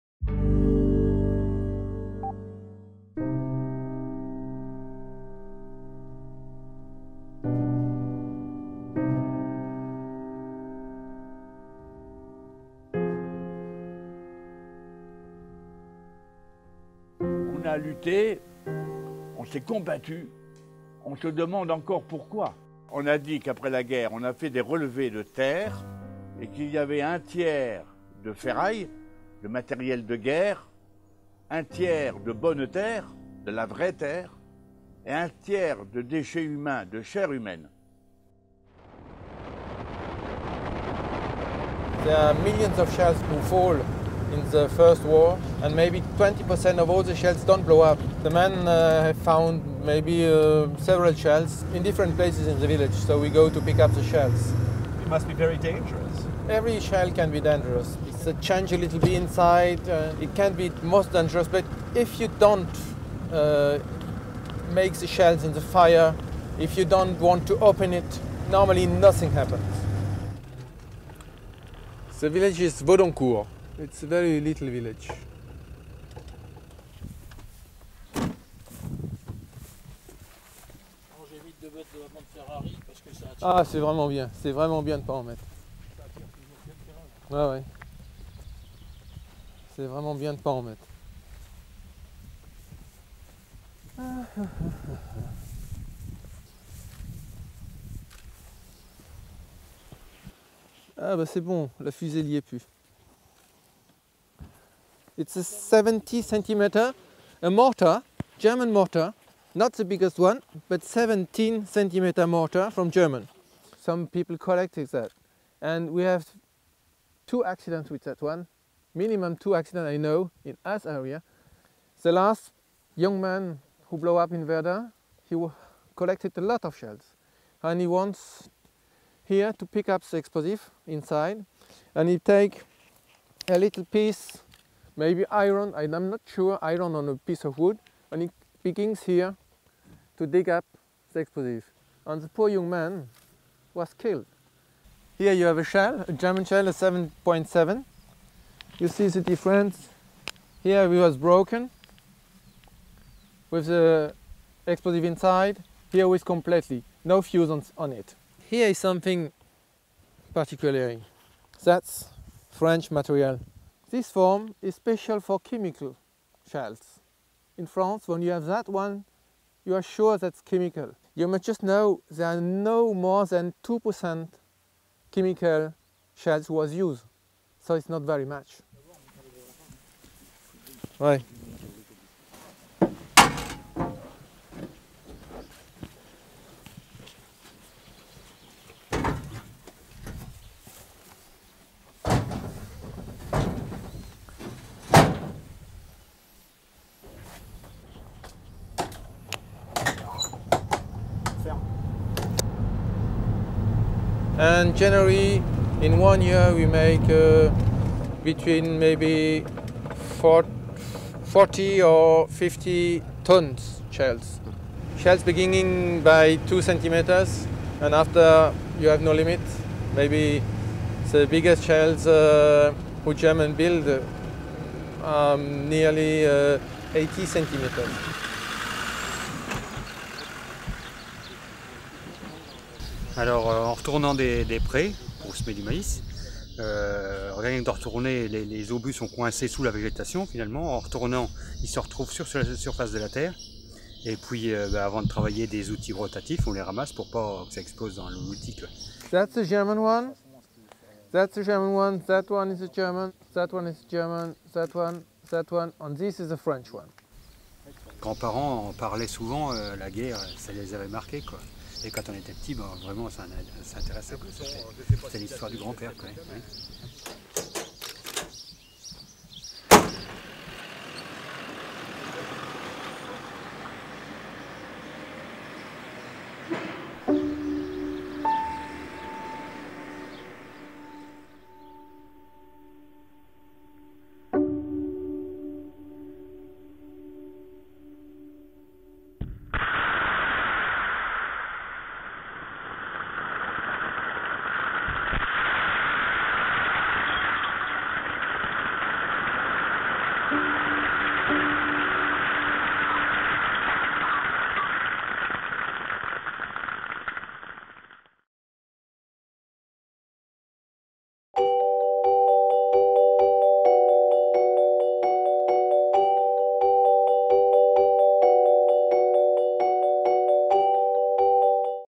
Destroying Bombs From World War I | The New York Times
France's Department of Mine Clearance is responsible for collecting and destroying unexploded shells still found by the thousands nearly 100 years after the Battle of Verdun.

Produced by: Erik Olsen